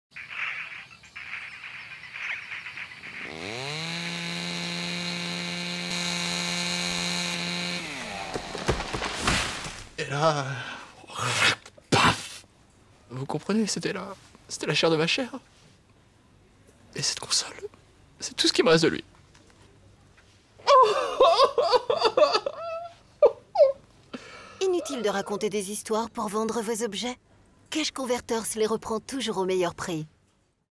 Spot TV Cash Converters 2018 - Les siamois #1
Retrouvez dans cette vidéo le nouveau spot TV de Cash Converters : Les siamois.